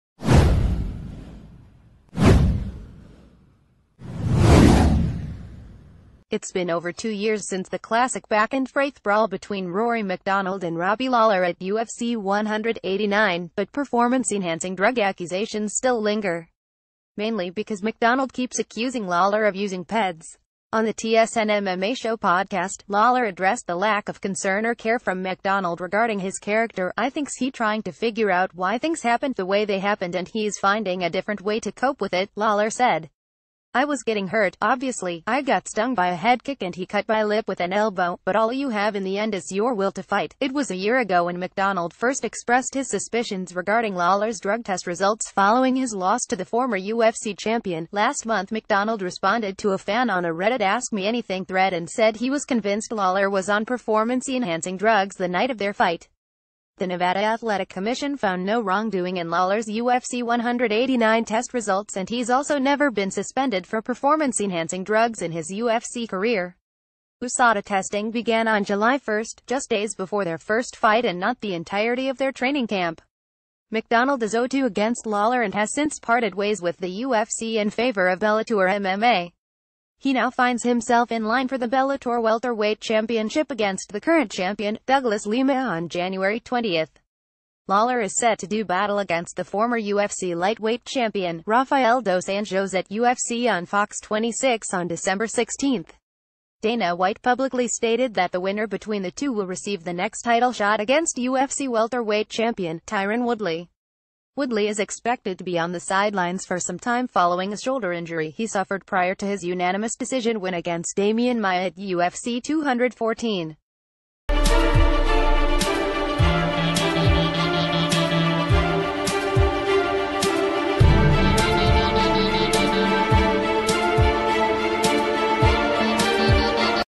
Breaking News | Robbie lawler responds to rory macdonald’s ped claims
Robbie lawler responds to rory macdonald’s ped claims

It’s been over two years since the classic backandforth brawl between Rory MacDonald and Robbie Lawl...